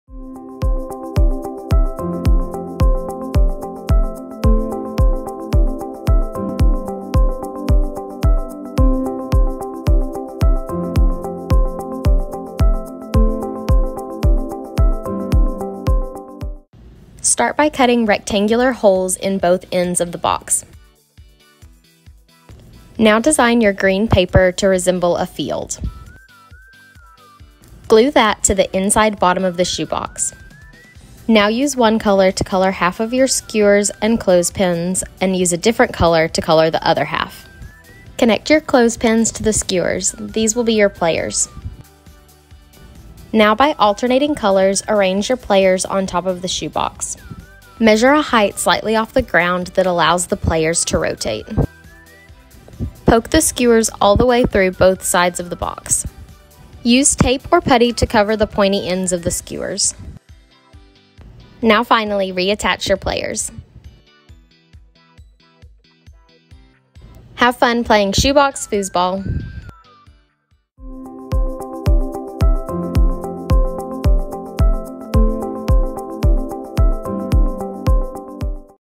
Shoebox Foosball
Instructor: Teresa Lucas, South Austin Recreation Center
Class name: Shoebox Foosball
Audience: Kids and Teens

Turn your old shoebox into a homemade version of a family friendly game: Foosball! With a shoebox and some craft supplies, have fun making and playing this new twist on a fan favorite!

Supplies:

- Shoebox
- 4 Wooden Skewers
- 10 Clothespins
- Table Tennis Ball
- 1 Sheet Green Construction Paper
- 2 markers
- White Crayon
- Tape